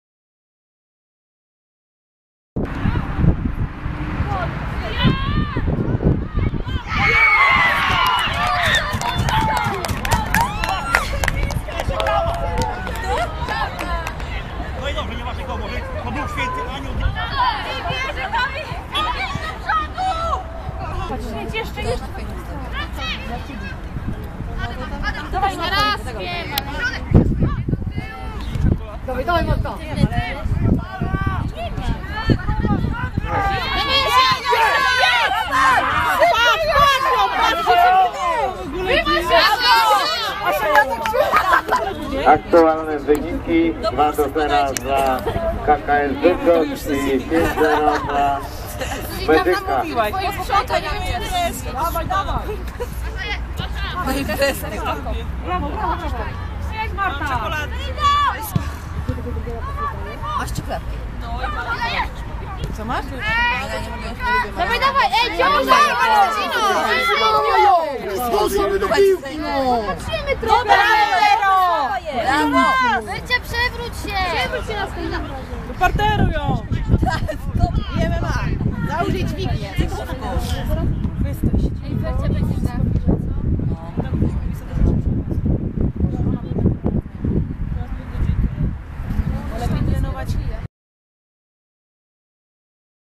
Turniej eliminacyjny Mistrzostw Polski Juniorek do lat 19 w Łodzi
Młode piłkarki Medyka Konin wygrały turniej turniej eliminacyjny Mistrzostw Polski Juniorek do lat 19, rozegrany w sobotę na boiskach UKS SMS przy ul. Milionowej w Łodzi.